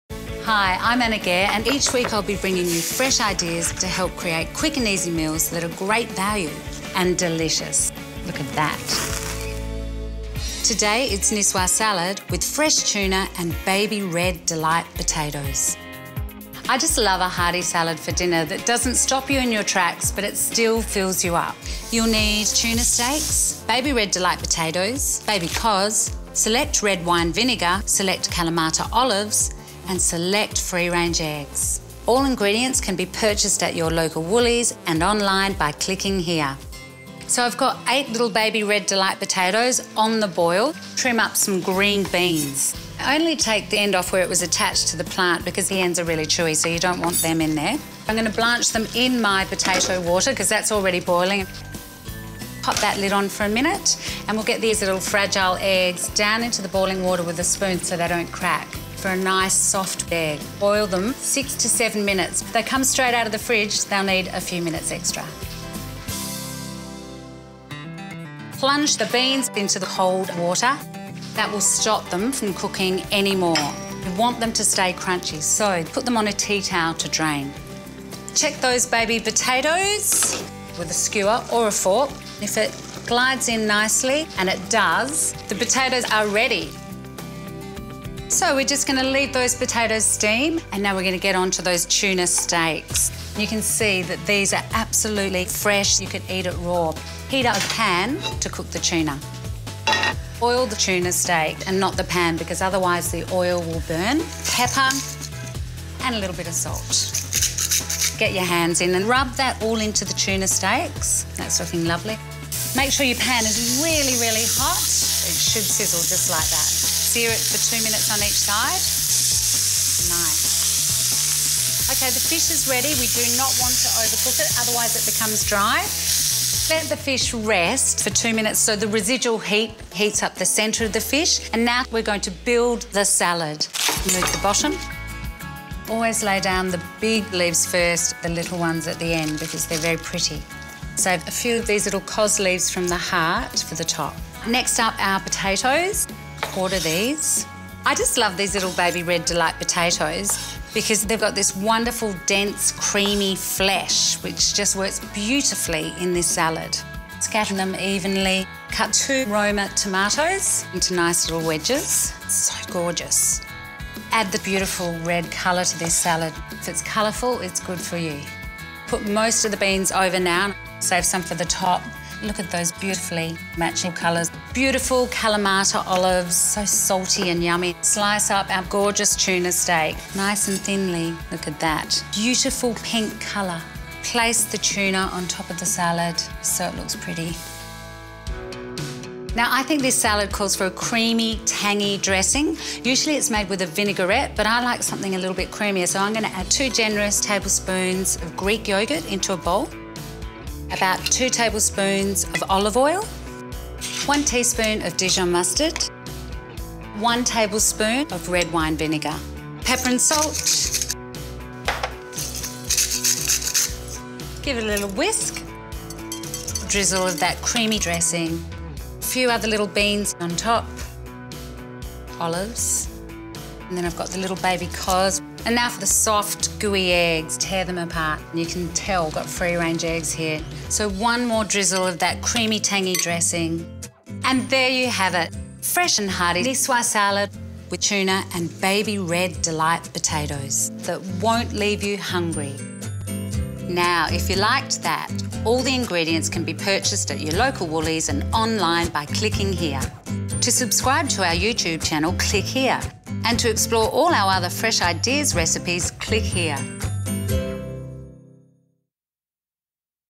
Nicoise Salad Recipe with Fresh Tuna & Baby Red Delight Potatoes | Woolworths
An easy tuna salad recipe that is a healthier lunch or dinner recipe. Swap tuna for salmon or chicken for your own salad twist.

Prep: 10 mins | Cook: 20 mins | Serves: 4

Ingredients:
8 baby red delight potatoes, unpeeled and halved
150g small green beans, topped and halved
4 Woolworths Select free-range eggs, at room temperature
3 small fresh skinless tuna steaks
1 baby cos lettuce
½ cup Woolworths Select pitted Kalamata olives
2 roma tomatoes, cut into wedges
½ cup coarsely chopped fresh continental parsley
Dressing
2 tablespoons Woolworths Select Greek yoghurt
1 tablespoon Woolworths Select red wine vinegar
2 tablespoons Woolworths Select Spanish Classic olive oil, plus extra to drizzle tuna
1 teaspoon Woolworths Select Dijon mustard

Method:
1. Cook potatoes in a large saucepan of boiling salted water for 12 minutes or until tender. Place the beans in a small colander and lower into potato water, cook beans for 1 minute or until bright green. Transfer to a bowl of cold water. Lower eggs on a spoon into a medium saucepan of boiling salted water and boil for 6-7 minutes. Drain. Refresh in cold water, peel and halve. Drain potatoes and let steam in a colander.
2. Heat a large non-stick frying pan over high heat. Drizzle tuna steaks with oil. Season. Cook tuna, turning once, for 1- 2 minutes each side or until cooked to your liking, transfer to a plate. Meanwhile, separate and wash cos lettuce leaves. Drain on a clean tea towel. Cut tuna into chunks.
3. Layer all the ingredients into 4 bowls, topping with tuna chunks and eggs. Whisk yoghurt vinegar, oil, and mustard in a small bowl. Season. Spoon over salad.

Tip 1: Use the Nicoise ingredients by thickly slicing potatoes, omitting egg and tuna to fill large baguettes spread with Dijon mustard and whole egg mayonnaise, then cut into pieces for vegetarian picnic rolls.
Tip 2: Substitute salmon fillets cooked to your liking or cooked barbecue chicken for tuna steaks.
Tip 3: Use a bamboo skewer inserted into the middle of potatoes to check if they are tender.